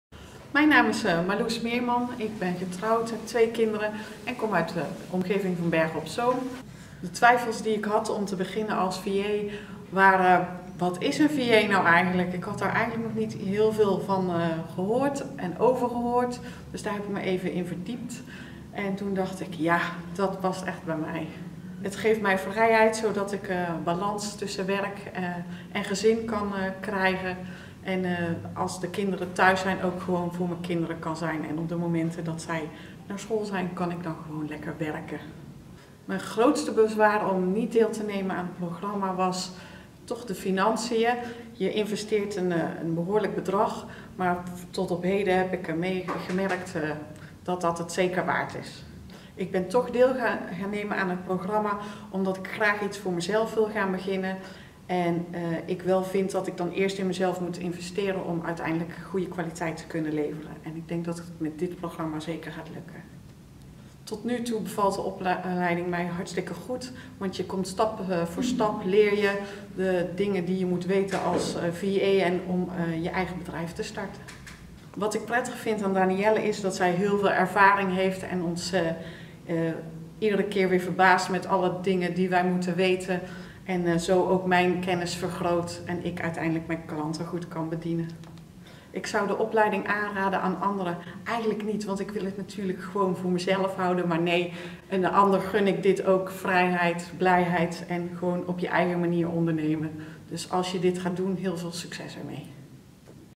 Marloes Meerman
Marloes Meerman vertelt hoe zij de opleiding 'Start succesvol als high level VA' heeft ervaren.

Wil jij ook een succesvolle start maken als virtueel assistent? Ga dan naar

VA Business Academy

Voor ambitieuze moeders die gaan voor een winstgevend bedrijf in eigen tijd als virtueel assistent.